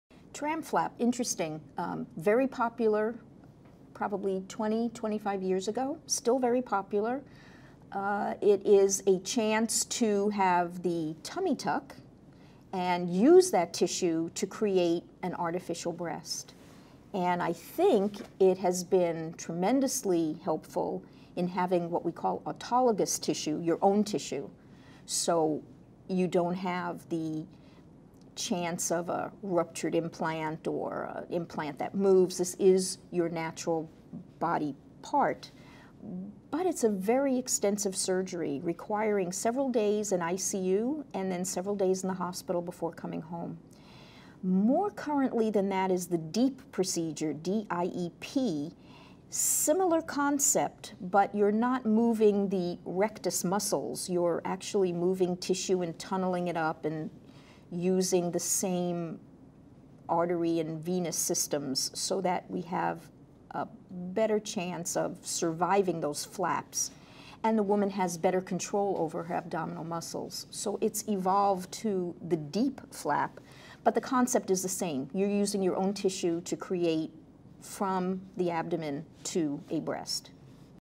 What is a TRAM flap procedure? - Cathy Cole, RN - Nurse Navigator
A transverse rectus abdominus (TRAM) flap procedure for cancer uses your own tissue to create a breast. In this video, Cathy Cole, a nurse navigator with Los Robles Hospital and Medical Center, discusses the pros and cons of this procedure.